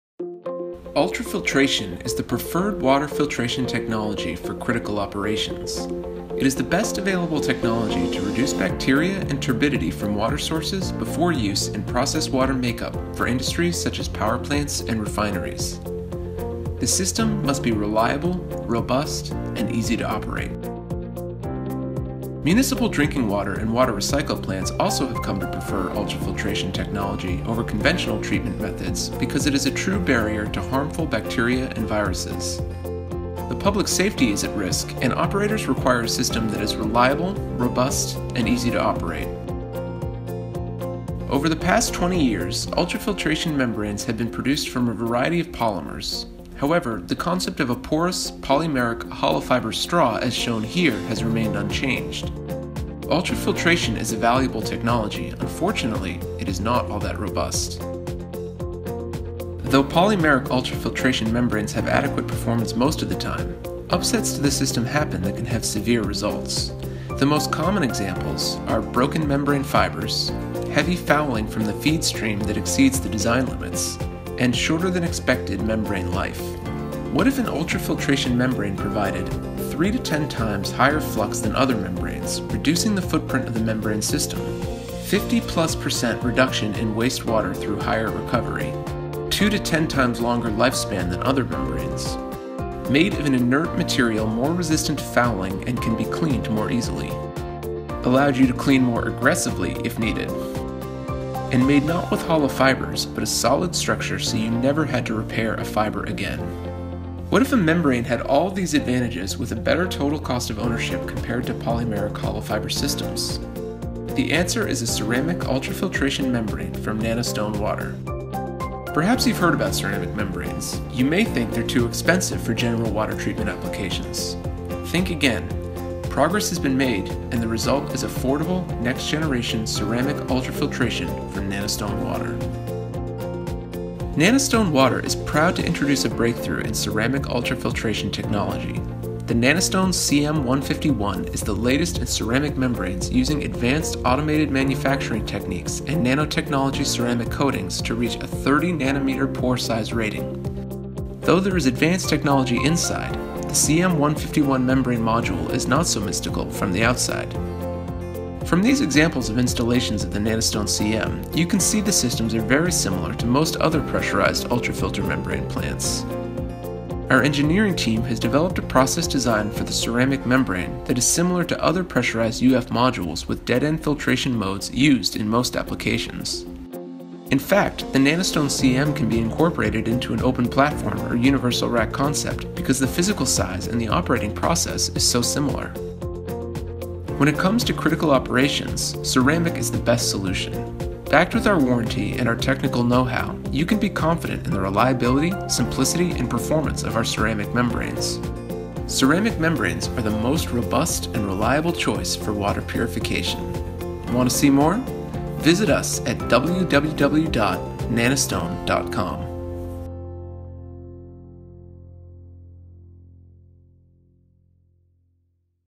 Nanostone CM Introduction
Learn more about Nanostone Water, Inc. as we highlight the key applications and benefits of our ceramic membrane technology.